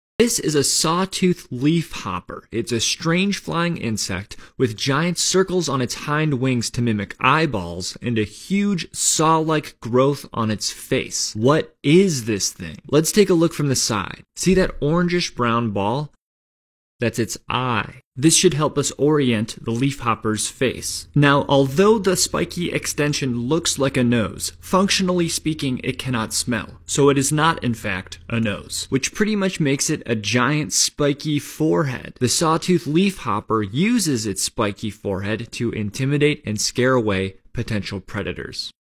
Sawtooth Leafhopper
A leafhopper is the common name for any species from the family Cicadellidae. These minute insects, colloquially known as hoppers, are plant feeders that suck plant sap from grass, shrubs, or trees.

And Stay In touch For This Type Of Videos